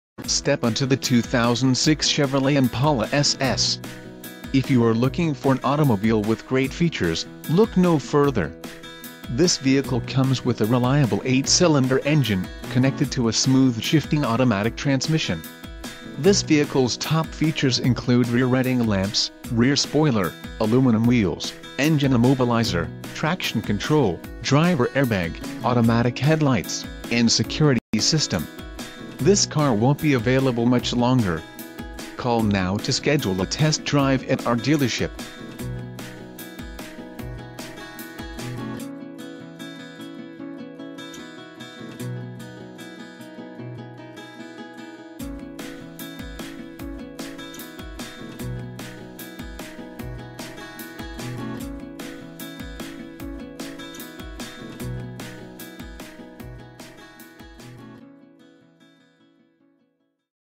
2006 Chevrolet Impala SS
This Vehicle is for Chevrolet fans the world over looking for a dependable jewel** Priced below NADA Retail!!! Why pay more for less! All Around gem! Safety Features Include: ABS, Traction control, Curtain airbags, Passenger Airbag, Front fog/driving lights...NICELY EQUIPPED: Power locks, Power windows, Auto, Air conditioning, Front air conditioning zones - Dual.